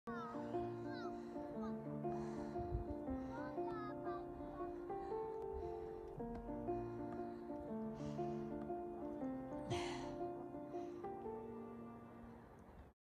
Pre-alps above Geneva Lake, Switzerland🇨🇭in my eyes
Overlooking the Lake Geneva from the hills of Montreux in Switzerland.